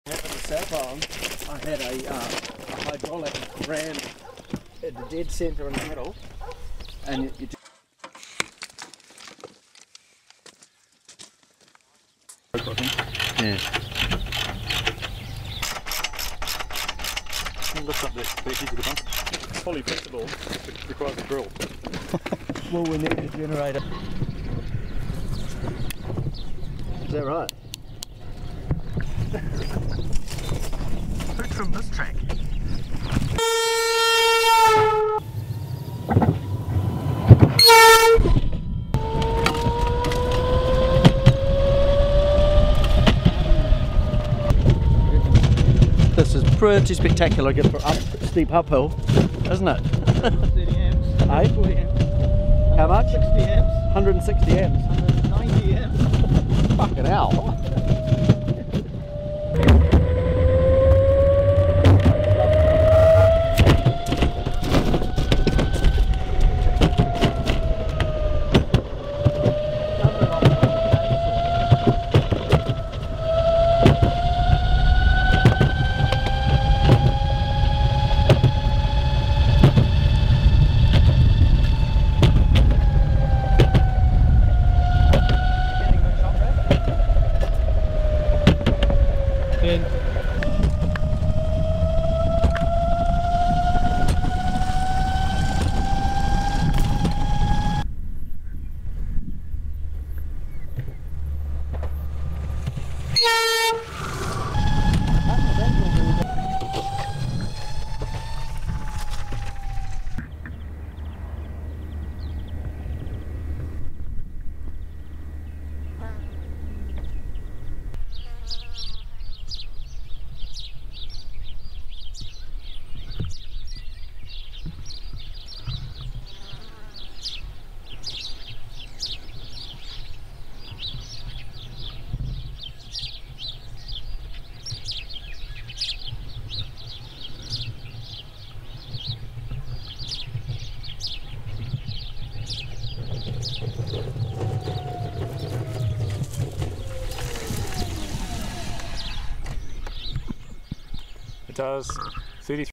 First test of our solar powered railway trolley
Testing our new electric railway trolley in New Zealand before taking it to abandoned rail lines around the world.